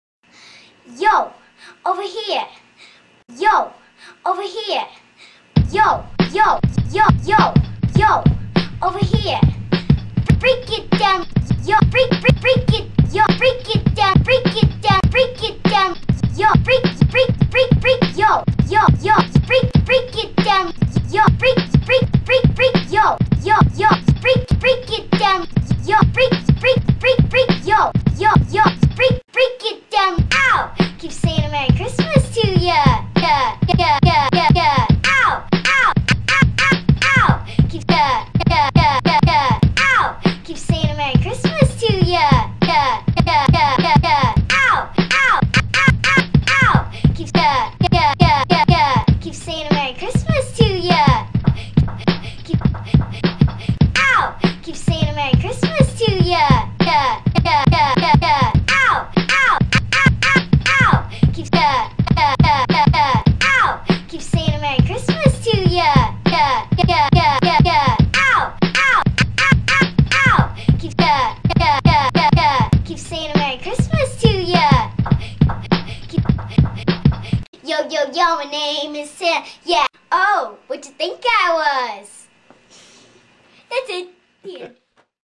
Santa Girl Rap (2007)
December 2007. This one is pretty embarrassing. Not very good at all, at all. Made with Windows Movie Maker but that's no excuse. No offense to the kid, it's just my editing here was basically atrocious. Oh well.